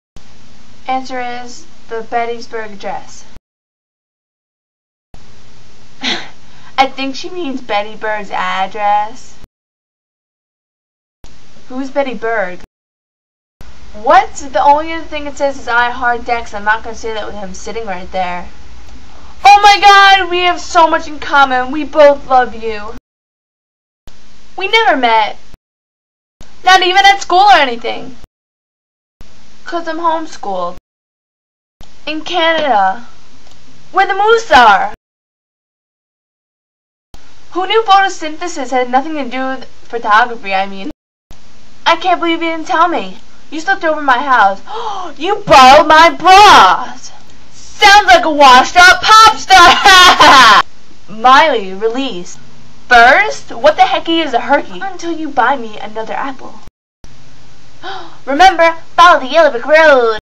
Emily Osment Voiceover Audition. :)
I'd love to be Emily Osment or Taylor Lautner! :)
But if those are taken... id be happy to be anything else! :)